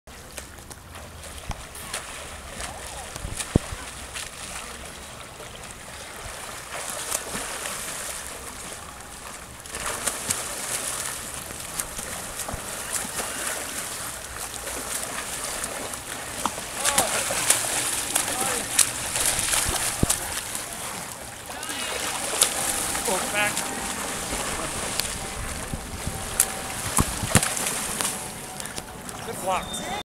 Masters Water Polo at Ammon - part 4
Water polo practice with Pgh Masters - casual footage.